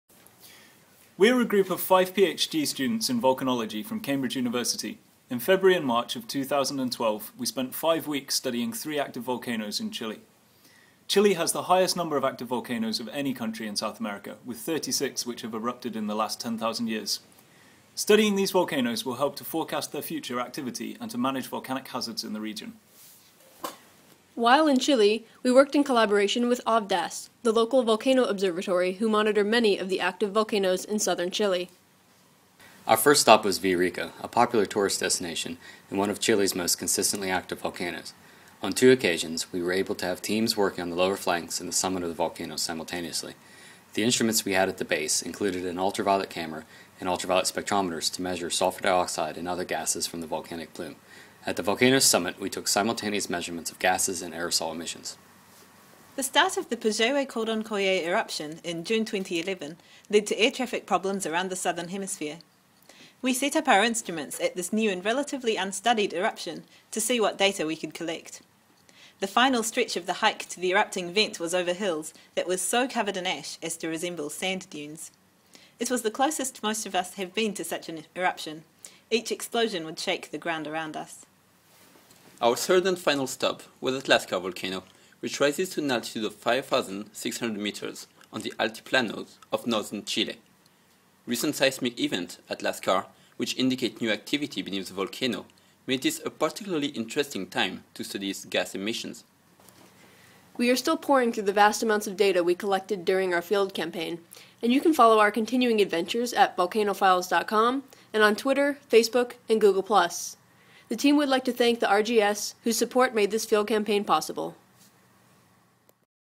Chile 2012 RGS Volcano Field Expedition Slideshow
Many thanks to the Royal Geographical Society for their sponsorship of our 2012 volcano monitoring campaign at Villarrica, Puyehue-Cordón Caulle, and Lascar volcanoes in Chile. This slideshow was created for use by the RGS-IBG.